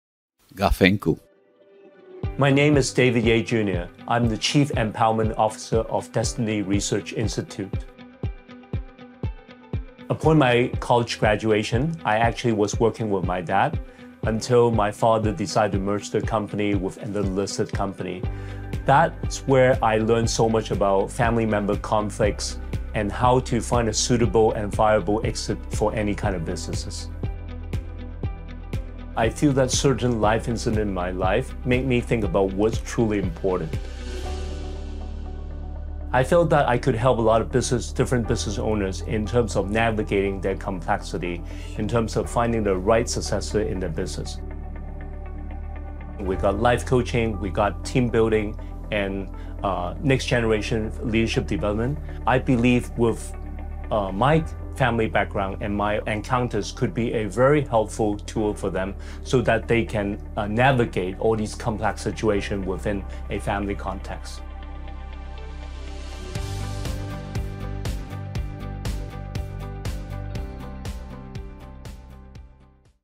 Mentoring maestro David Yeh Jr sets the businesses of other prominent families to rights
David Yeh Jr had tough love from his hugely successful father, and his knowledge gained throughout his business career inspired him to set up his coaching company and help others.

WeChat: Gafencu

Videographer: Jack Fontanilla